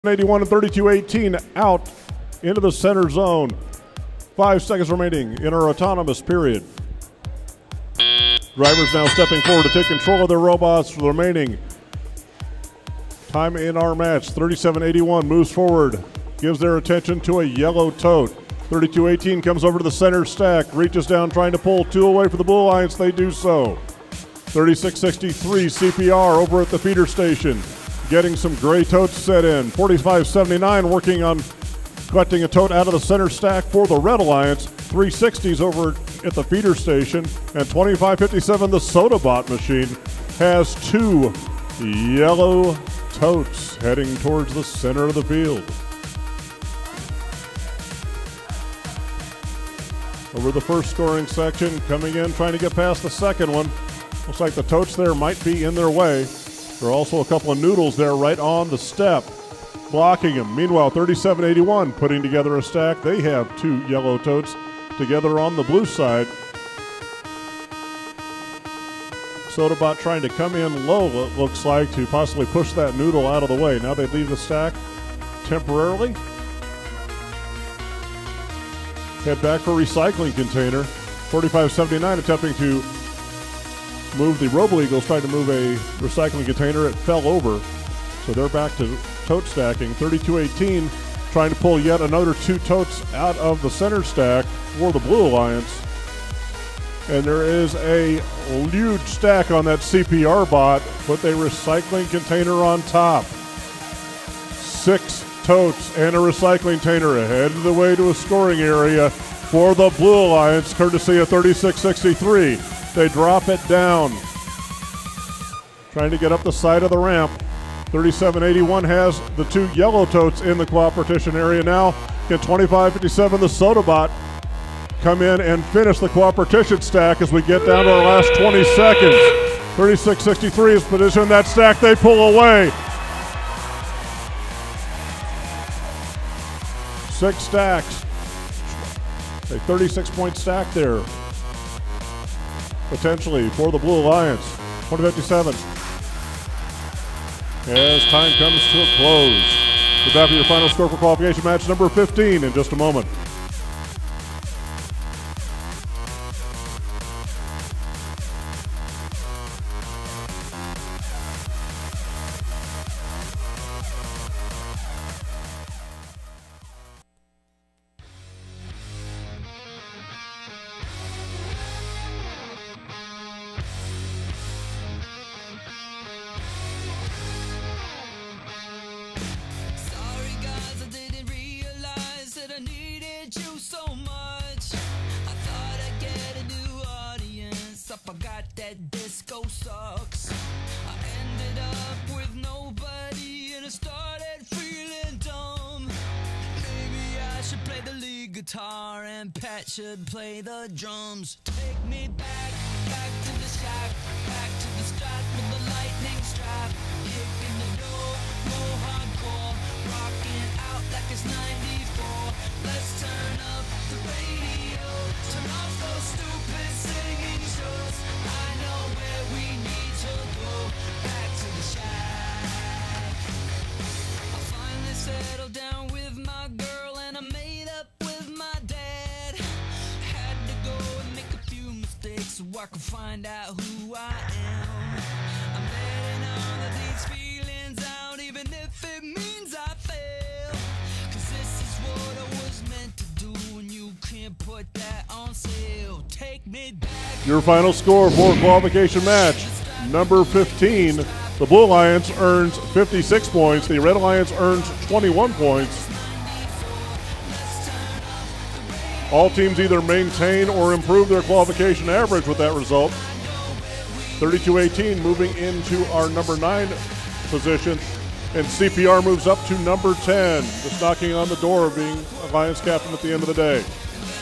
2015 PNWFIRST Auburn Mountainview Qualifying Match 15
2015 PNWFIRST Auburn Mountainview Match 015 courtesy of PNW Media Crew (PNWFIRST.org)!

Six tote stack with a container!